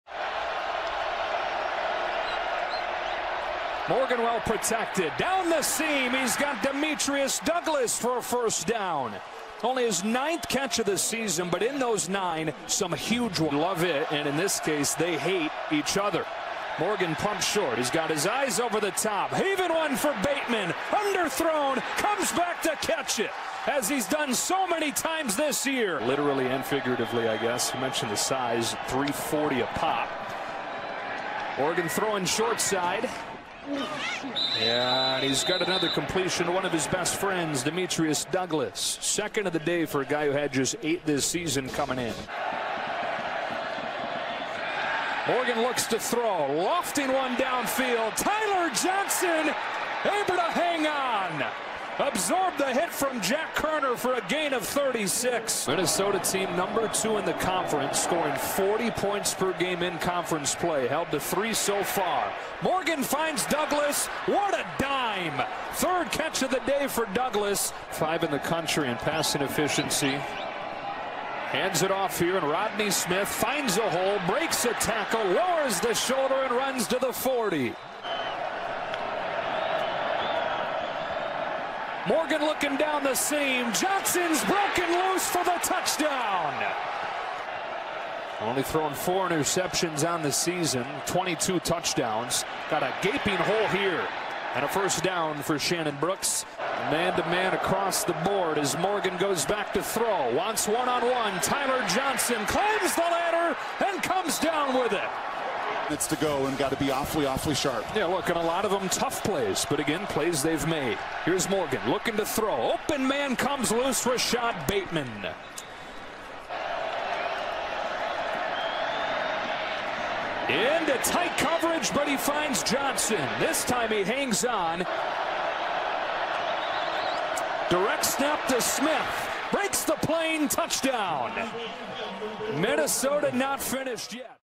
Gopher Football Offensive Highlights vs. Iowa Hawkeyes
Watch Gopher Football's best offensive plays vs. Iowa on Nov. 16, 2019.